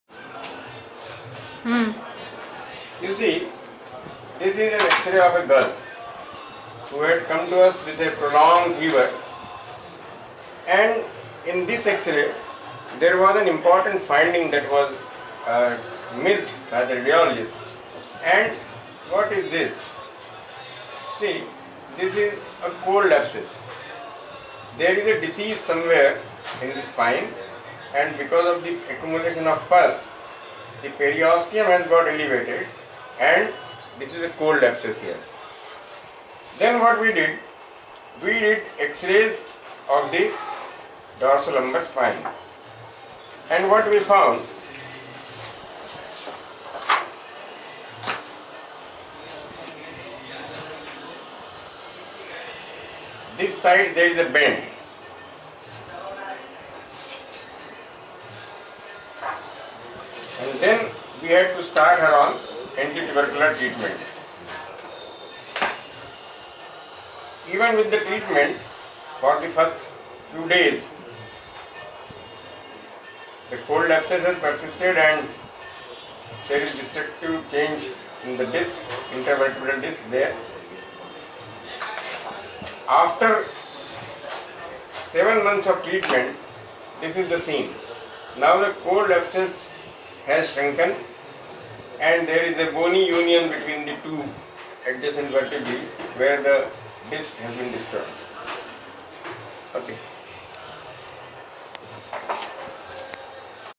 Pott's spine with cold abscess
X-Ray views of a case of Pott's Spine with a cold abscess. In this case the anti-tubercular treatment has led to a complete resolution and reabsorption of the pus of the cold abscess.